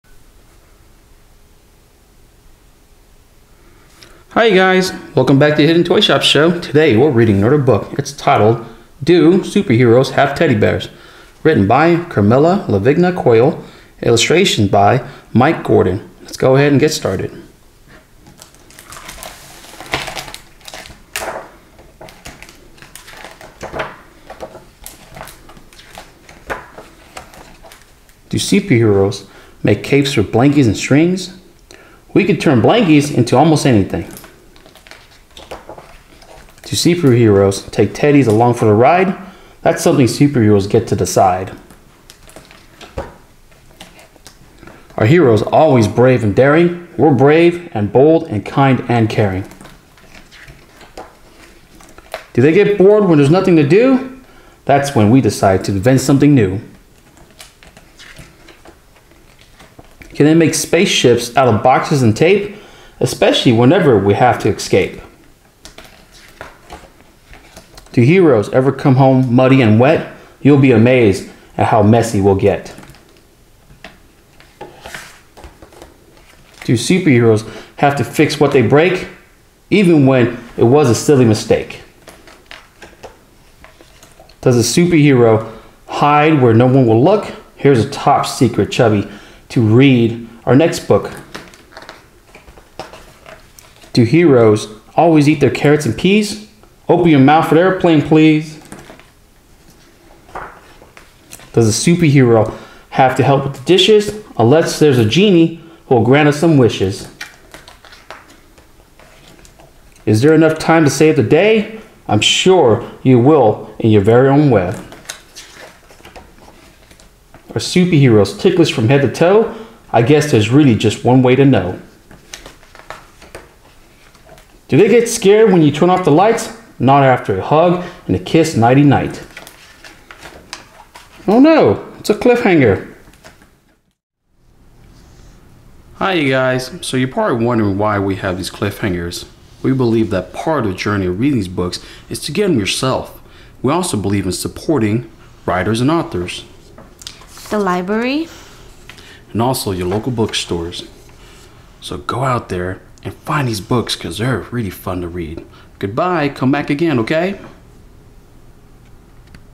Do Superheroes Have Teddy Bears? - Read Aloud Storybook For Kids - Bedtime Stories - Cliffhanger
Welcome to The Hidden Toy Shop Show.  Today we are reading experts from a wonderful book titled "Do Super Heroes Have Teddy Bears?" written by Carmela LaVigna Coyle and illustrated by Mike Gordon.  FYI: We only read small parts from the book and include a Cliffhanger.  We believe that writers, authors, libraries and also book stores should be fully supported so we encourage all readers to go out and finish this book.  Come back anytime!!!